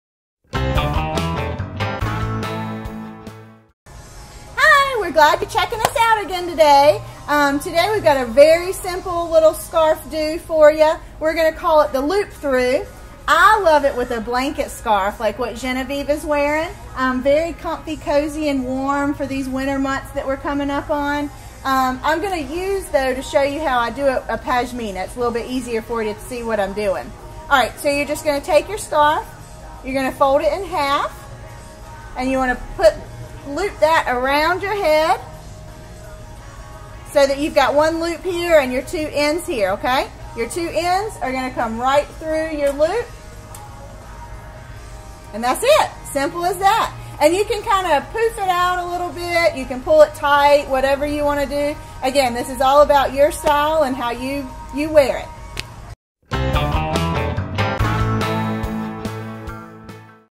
How To Tie a Scarf 30 Ways: The Loop Through
Today's video shows how to wear your scarf in a loop throgh. Brandi demonstrates how easy it is to tie and we're sure you'll love it.
How do you tie your scarf? We'd love to hear from you! Let us know below or on Facebook